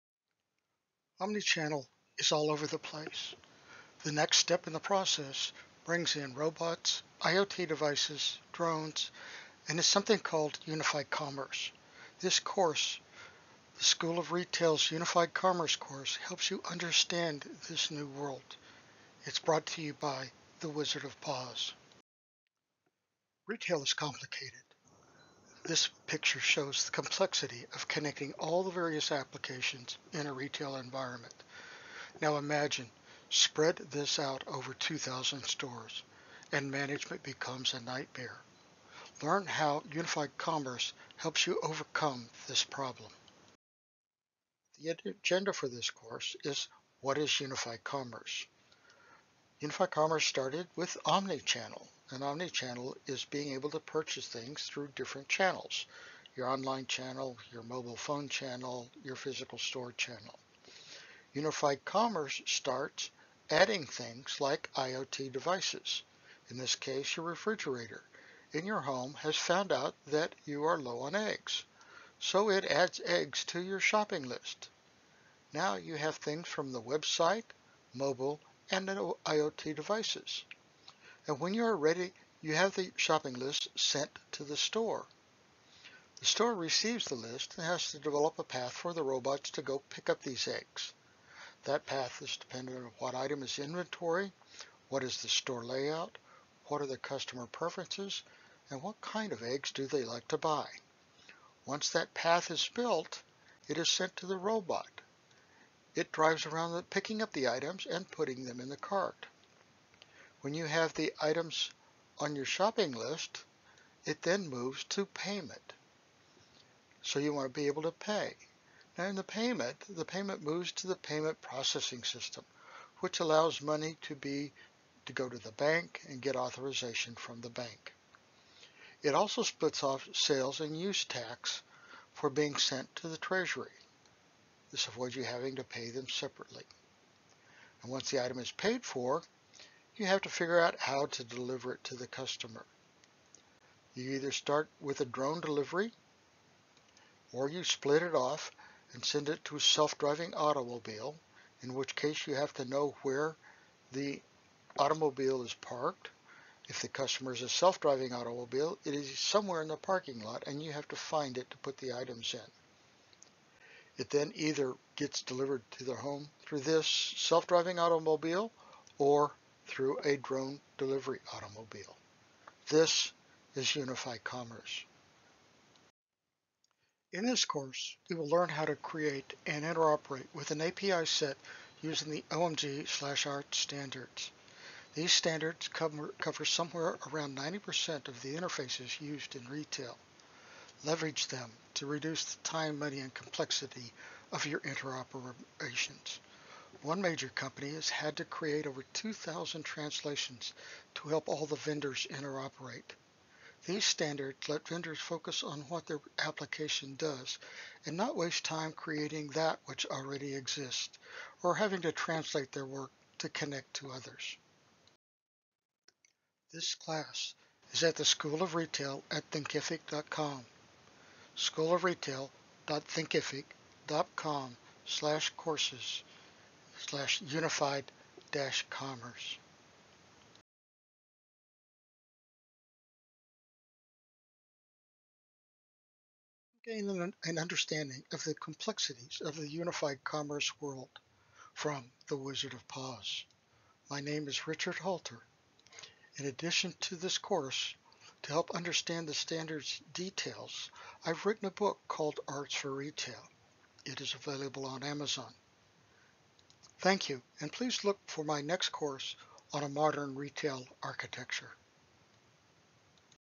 What is Unified Commerce
What is Unified Commerce?  How does it relate to Omni-Channel?  How can I leverage that which already exists?  Learn at the School of Retail - Unified Commerce course.